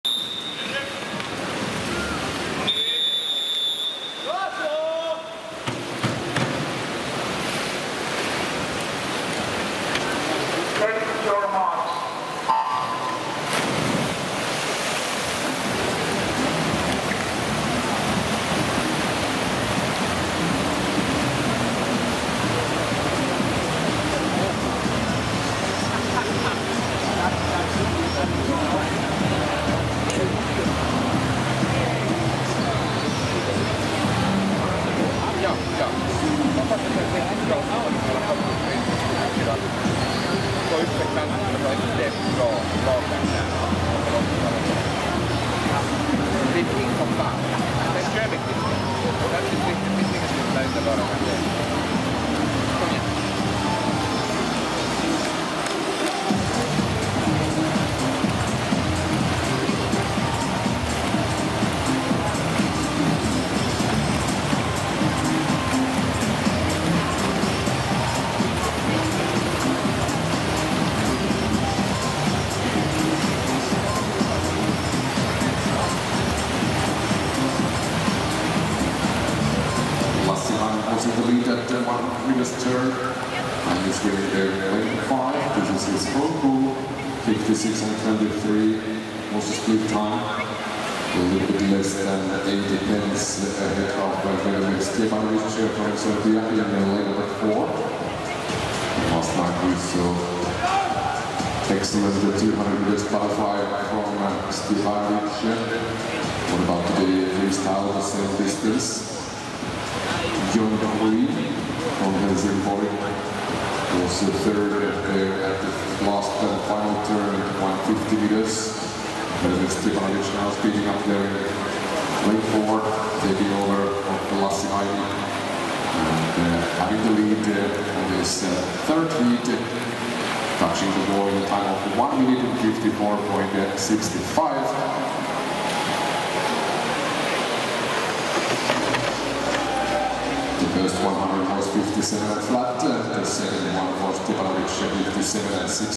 Speedo Helsinki Swim Meet 2022: Event 18, 200m Freestyle, Men, Heat 3
Lane 6 Felix Hofmann